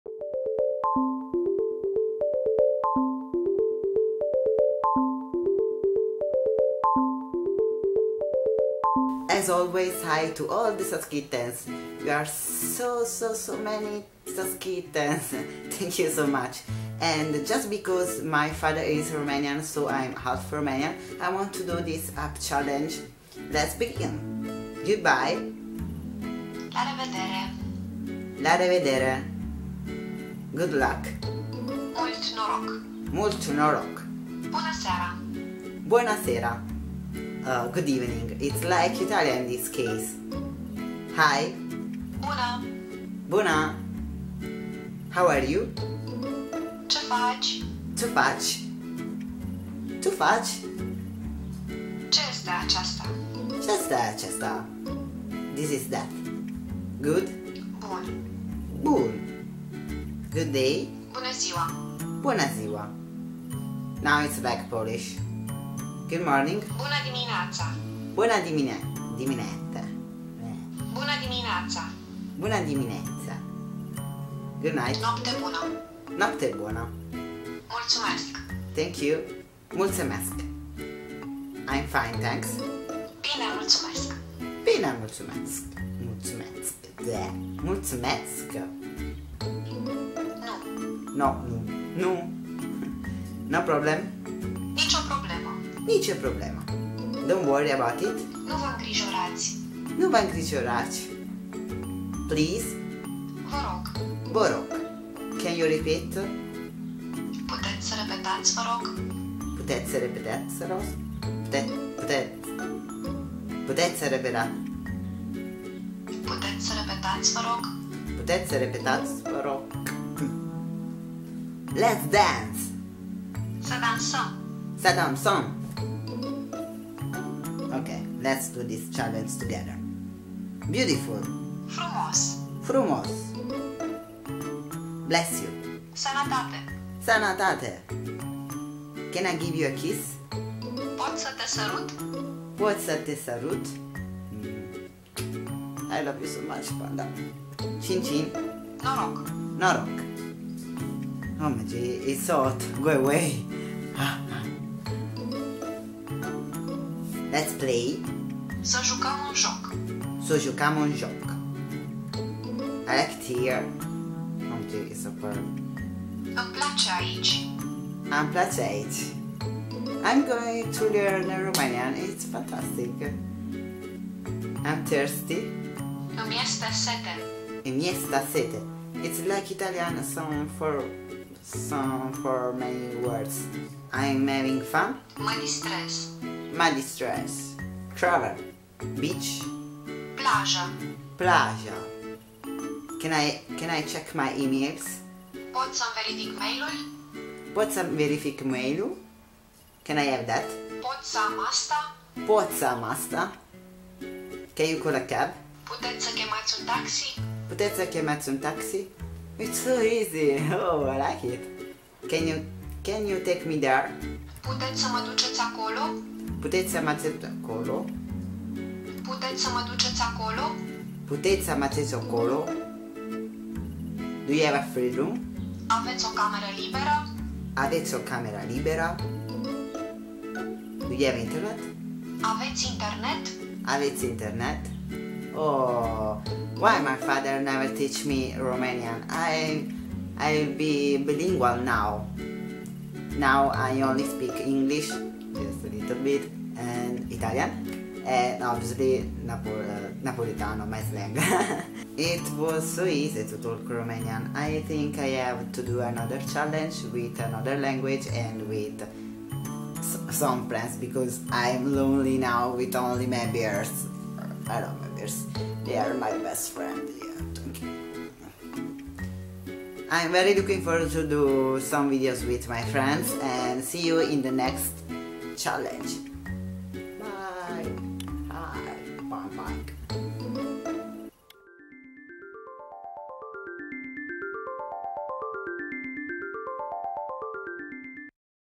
Language Challenge: Romanian
I'm half blood Romanian but my father never teach me it!
So I want to try!